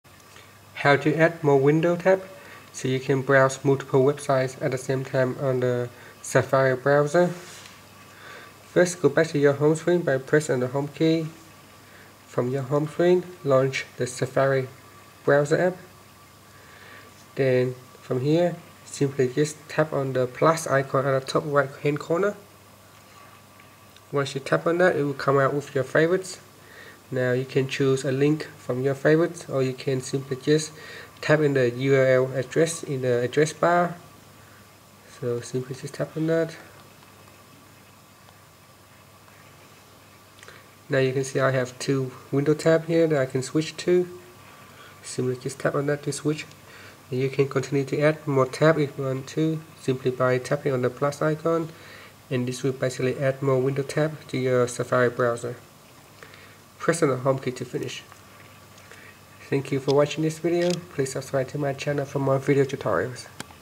iPad Mini iOS 7: How to Add More Window Tab on Safari Browser
Learn how you can add more Window tab on Safari browser of the iPad Mini iOS 7.